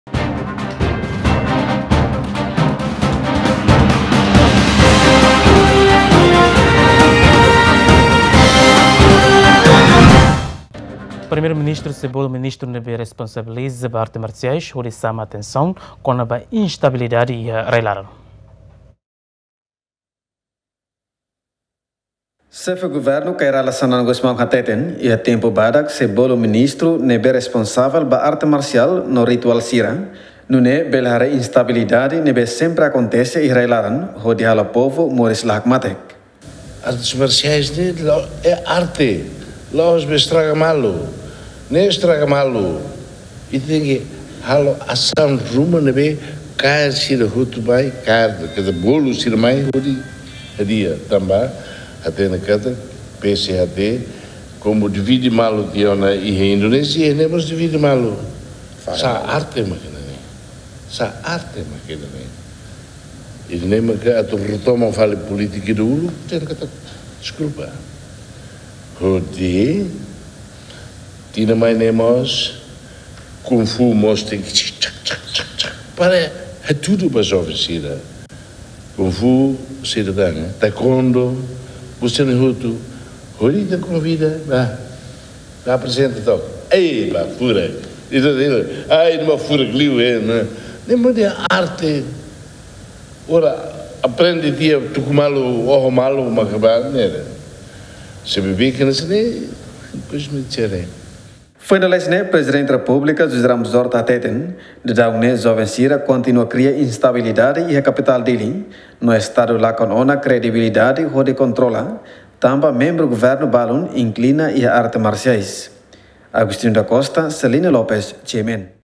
PM Xanana sei bolu Ministru ne’ebé responsabiliza ba Arte Marsiais
Primeiru Ministru Xanana Gusmão sei bolu Ministru ne’ebé responsabiliza ba Arte Marsiais